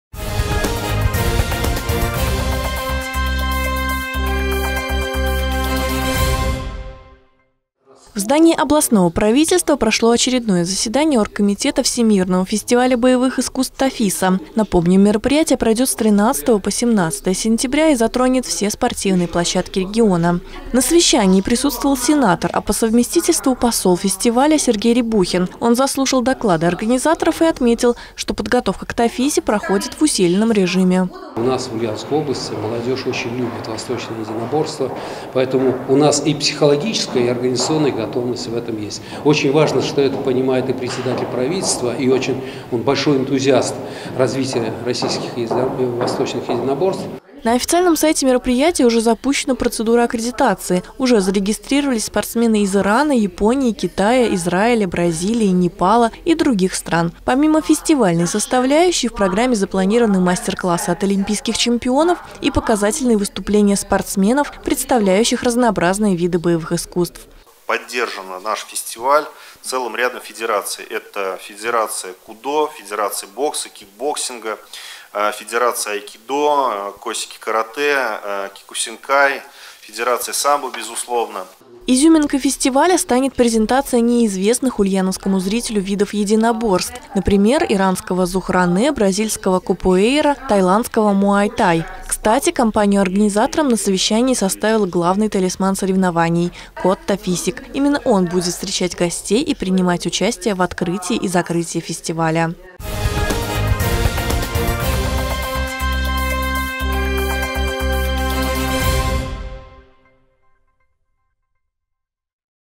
Ульяновцы увидят национальные единоборства Бразилии, Ирана и Тайланда
В областном правительстве прошло очередное заседание оргкомитета I Всемирного фестиваля боевых искусств «ТАФИСА». Напомним, мероприятие пройдет с 13 по 17 сентября и затронет все спортивные площадки региона.